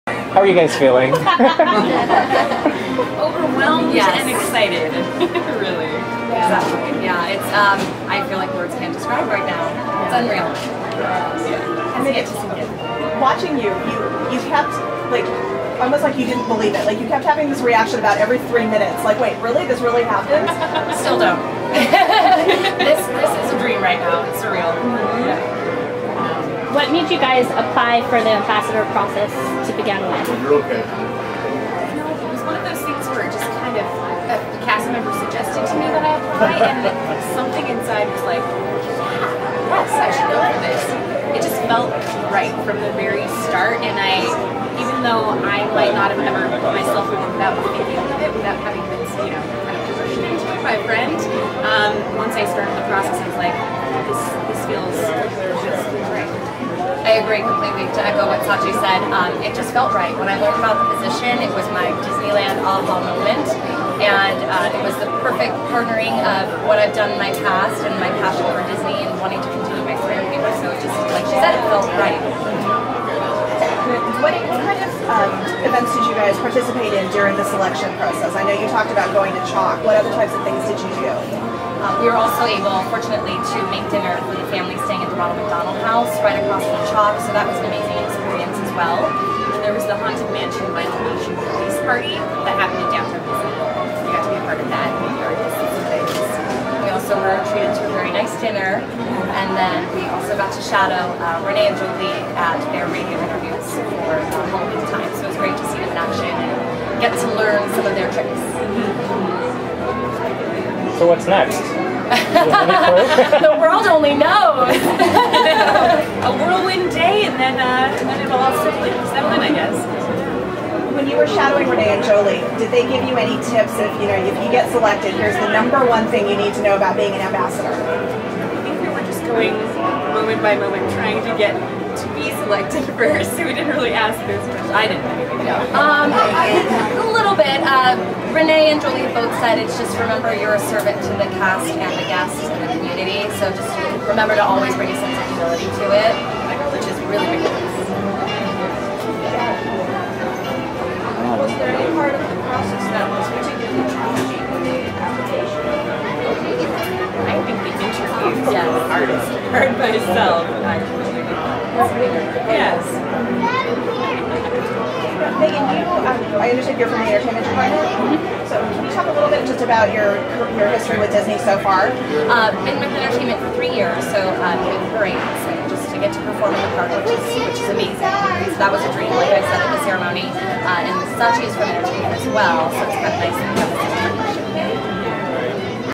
Interview with New Disneyland Resort Ambassadors - October 8, 2012
On October 8, 2012, Disneyland Resort President George A. Kalogridis announced the Disneyland Resort Ambassador Team for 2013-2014. The two cast members chosen for this team were Megan Navarette and Sachiko White, both of the Entertainment department at the Disneyland Resort. Here is an interview with the new Disneyland Resort Ambassador Team.